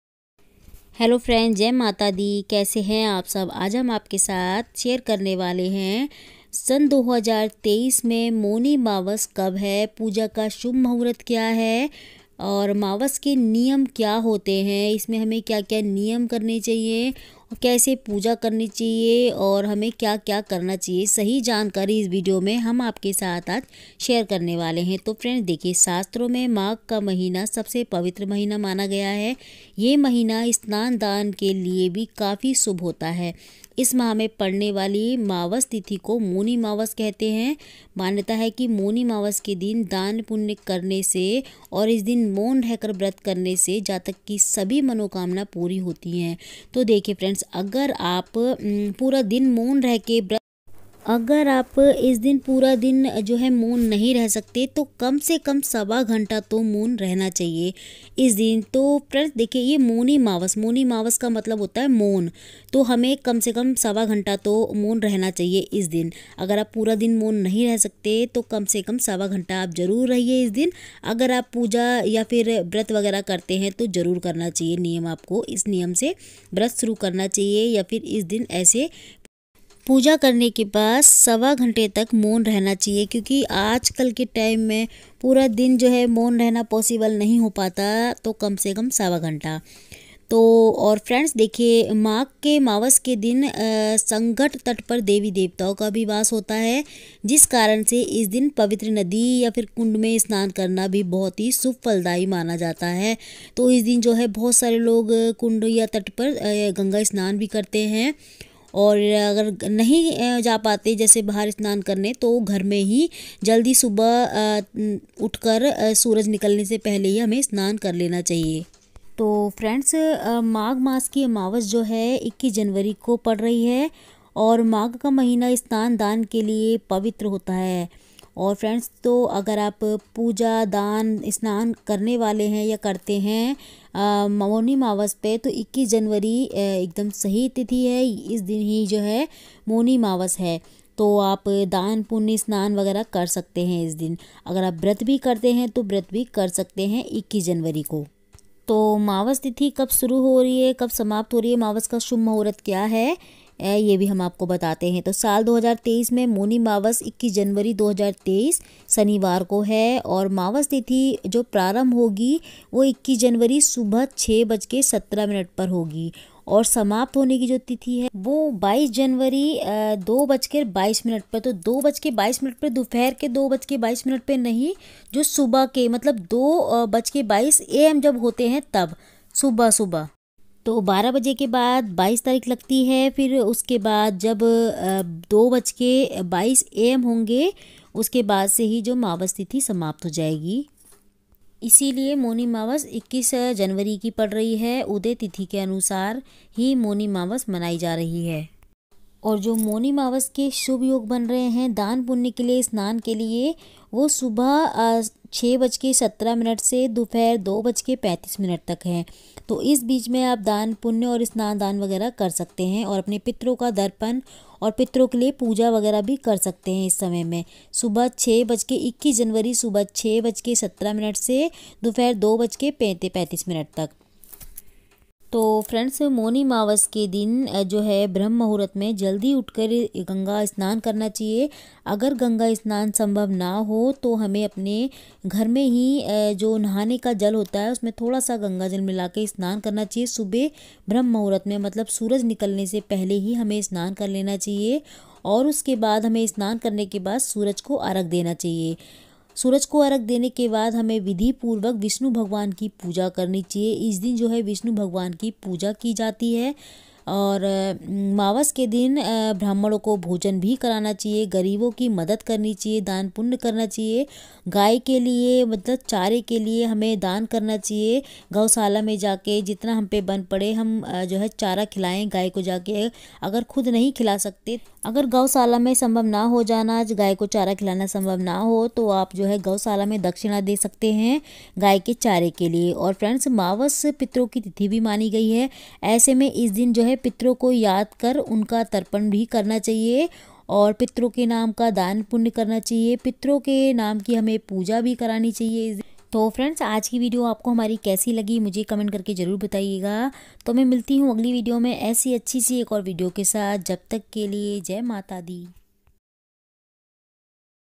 2023 मैं मौनी अमावस्या कब है | साल की सबसे बड़ी मौनी अमावस्या शुभ मुहूर्त पूजा विधि नियम क्या है
2023 मैं मौनी अमावस्या कब है | साल की सबसे बड़ी मौनी अमावस्या शुभ मुहूर्त पूजा विधि नियम क्या है |

hello friend || Jay Mata Di kaise hain aap sab... Aaj Ham aapke sath share kar rahe hain Saal 2023 me moni amavasya kab hai... Moni amavasya ka Shubh muhurt kya hai Puja Vidhi Vrat ke Niyam sampurn Jankari Humne is video mein aapke sath share Ki Hai... Moni amavasya par  Puja Kaise Karni chahiye kya Dan Punya karna chahiye...moni amavasya par hame mon rahna chahiye niyam ka palan karna chahiye...